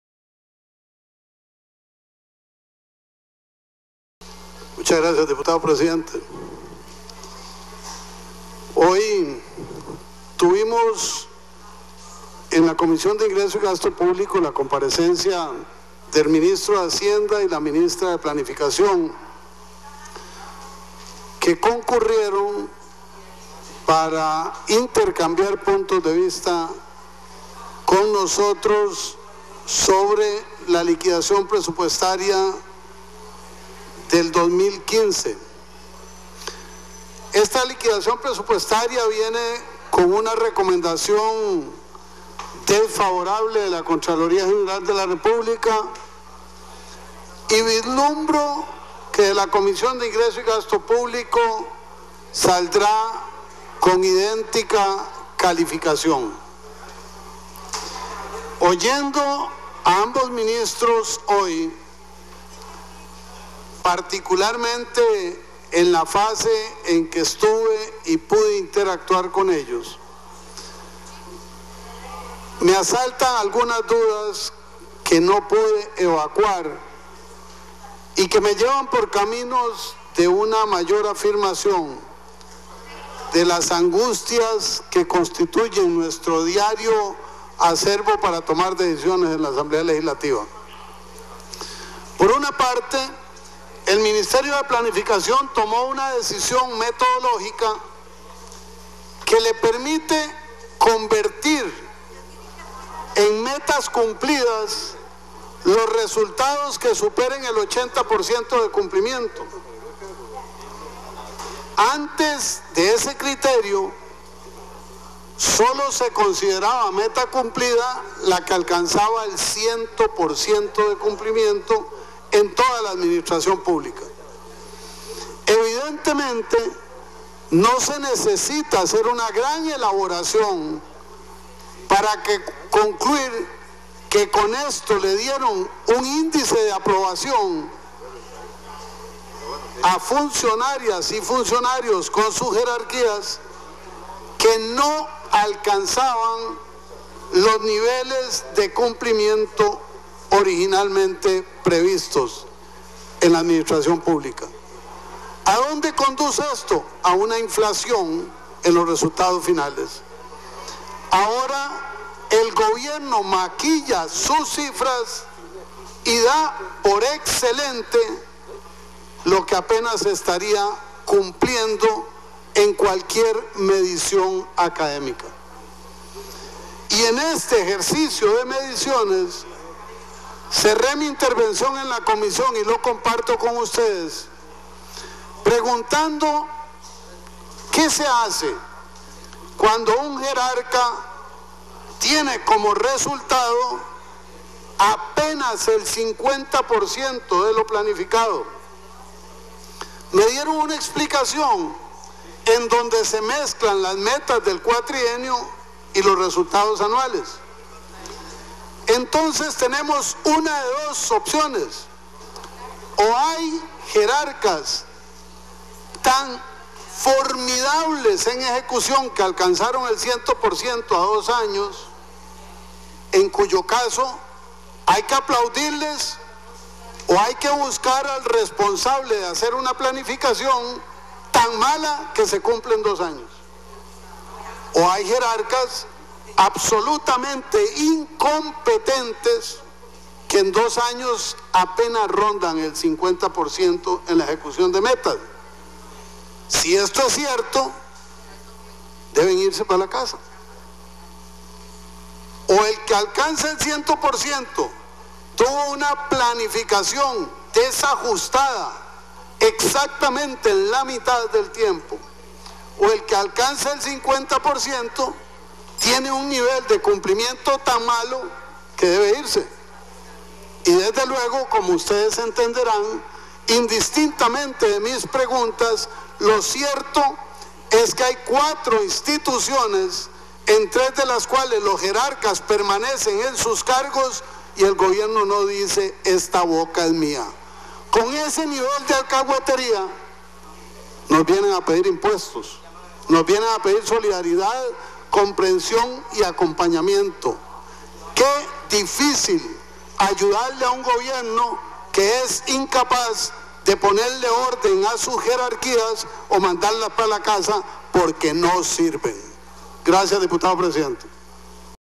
Rolando González alerta liquidación del Presupuesto recibió criterio desfavorable de Contraloría
Posterior a la comparecencia de autoridades de la Contraloría General de la República, ante la Comisión de Asuntos Hacendarios, el diputado liberacionista alerta sobre el criterio desfavorable que esa institución brindó sobre la liquidación del Presupuesto, también afirmó que hubo una inflación en los resultados finales en las metas del gobierno en lo que llevan ejecutado del PND

“Terminamos preguntando ¿Qué se hace cuando un Jerarca tiene como resultado apenas un 50% de lo planificado, tenemos 1 de 2 opciones, o hay Jerarcas tan formidables que alcanzaron el 100% en dos años, o hay,  incompetentes que en dos años apenas llegan al 50%, con esto, queda difícil de ayudar a un Gobierno que es incapaz de poner en orden a sus jerarquías o mandarlas para la casa porque no sirven”